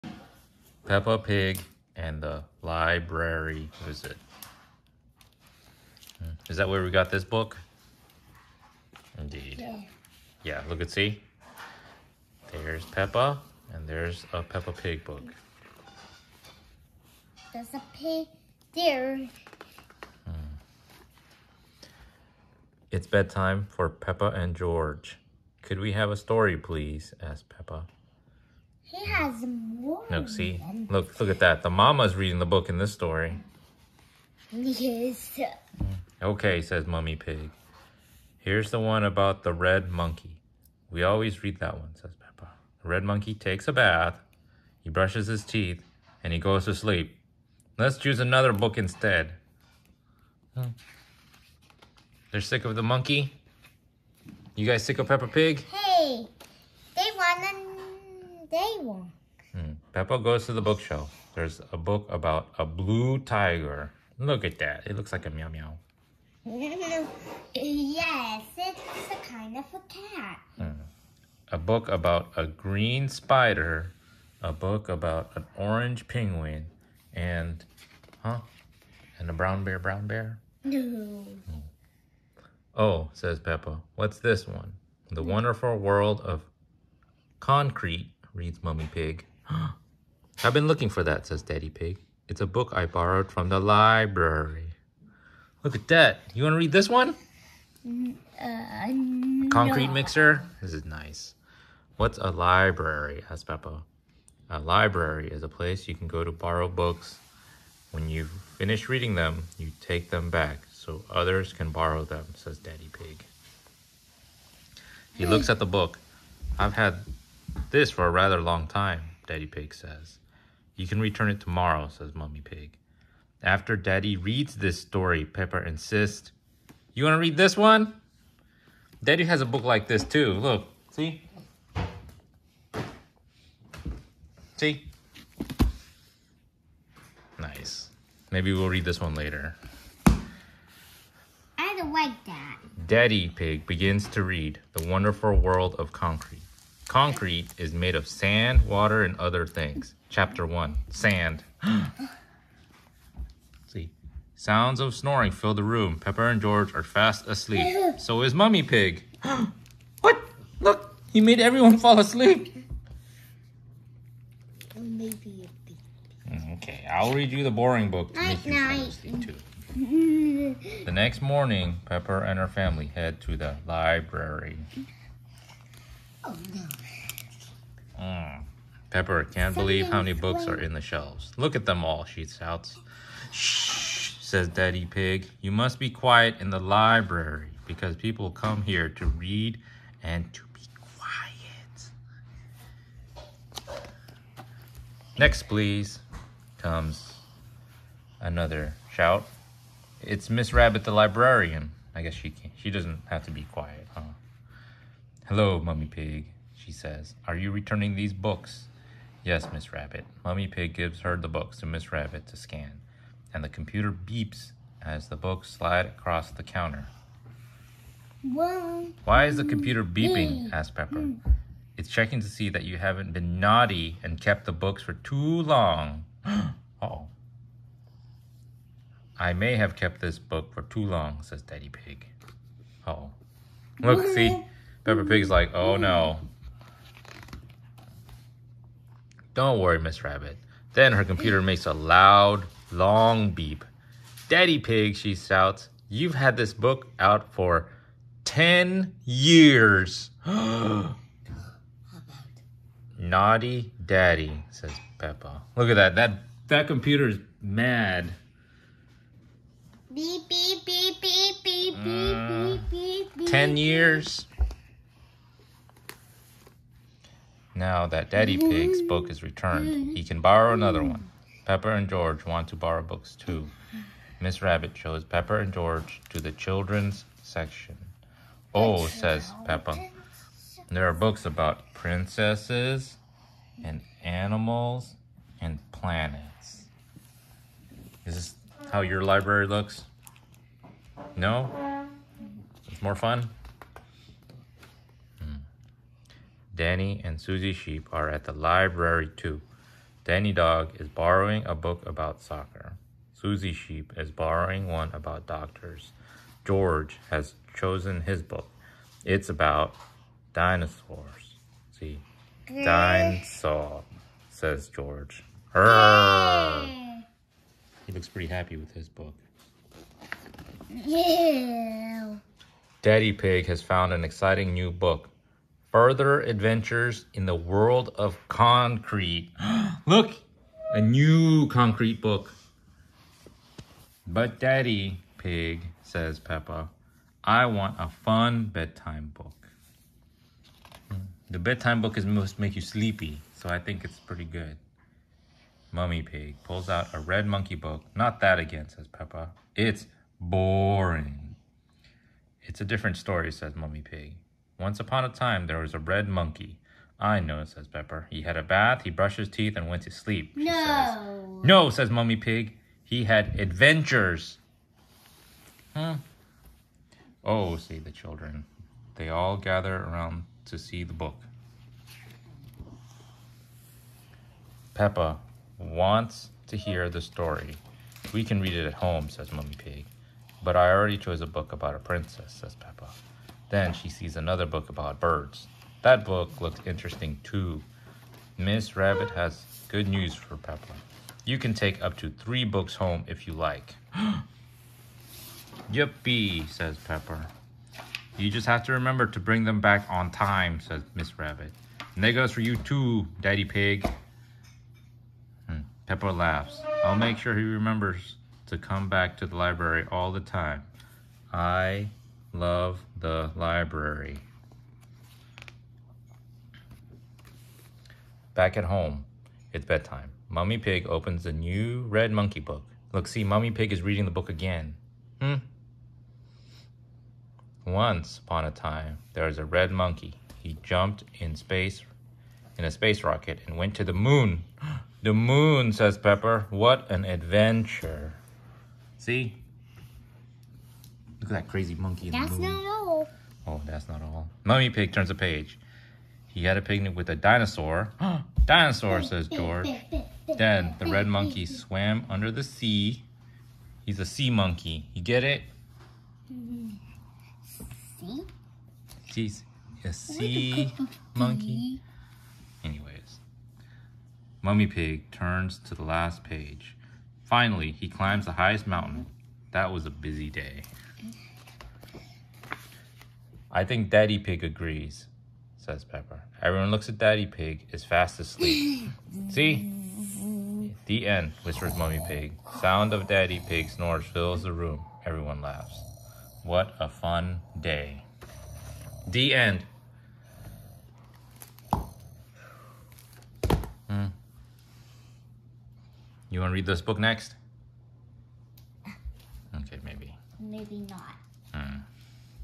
Peppa Pig and the Library Visit (Read Aloud) (Read Along) 2023
Peppa Pig and her brother, George, discover the wonders of the public library.

Peppa and George are tired of all their regular bedtime stories. The only book they haven’t read is Daddy’s library book, The Wonderful World of Concrete . . . what a snooze! It’s overdue anyway, so the family sets out for Peppa’s very first visit to a library. With so many books to choose from, how will Peppa ever decide which ones to borrow? Bedtime will never be boring again!